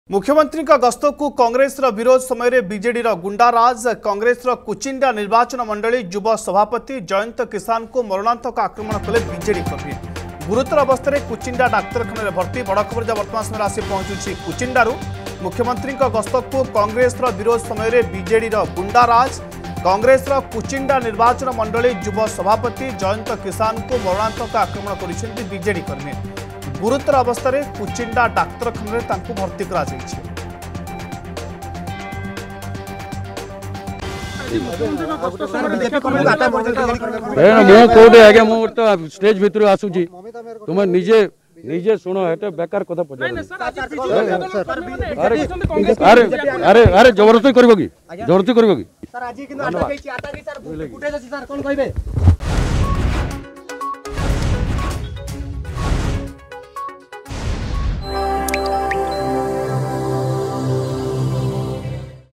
ନଜର ପକାନ୍ତୁ ଏ ଦୃଶ୍ୟ ଉପରେ ।
ନଜର ପକାନ୍ତୁ ଏ ଦୃଶ୍ୟ ଉପରେ । ସାମ୍ବାଦିକ ମାନେ ପ୍ରଶ୍ନ କଲେ କେମିତି ବ୍ୟବହାର କରୁଛନ୍ତି  ମନ୍ତ୍ରୀ ସୁଶାନ୍ତ ସିଂ ।